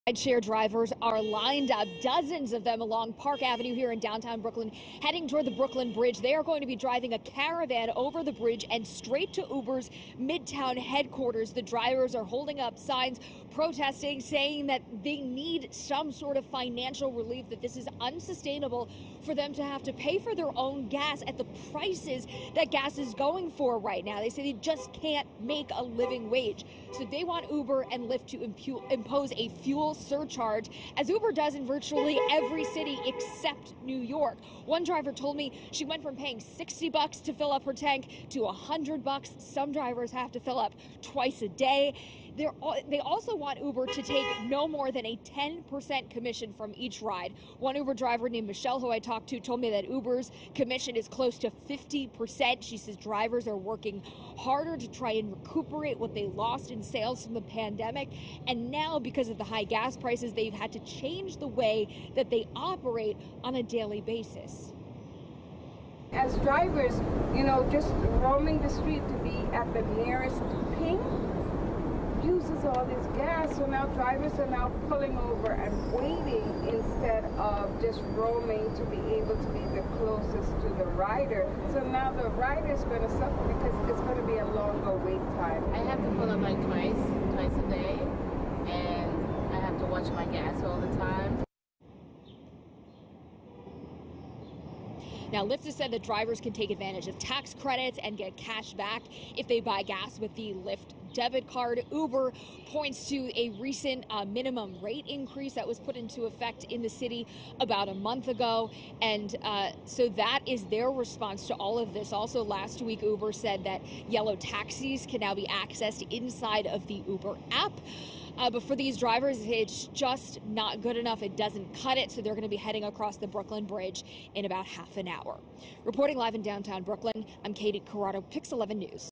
NYC rideshare drivers stage caravan, demand added surcharge to offset gas prices
On Tuesday, the Justice for App Workers coalition is caravanning across the Brooklyn Bridge to Uber’s Midtown offices. The coalition represents 100,000 New York drivers and delivery workers.